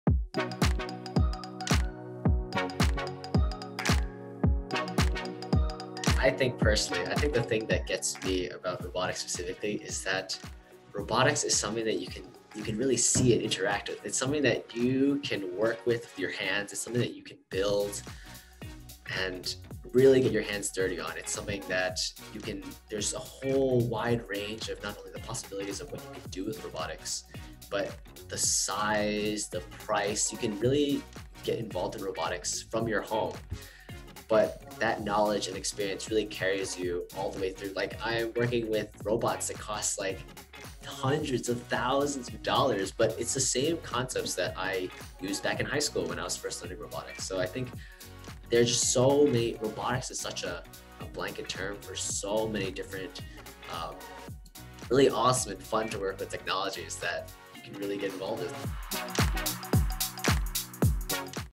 Change the Game with Robotics | National Robotics Week | iD Tech
It’s National Robotics Week, a perfect time to get those gears turning and learn more about one of the most exciting areas of STEM. Build. Engineer. Create. iD Tech Rockstar Instructor James Chin shows how learning the fundamentals of robotics at a young age can lead to exciting careers and fascinating college majors. Get started with robotics today and explore its limitless possibilities! iDTech.com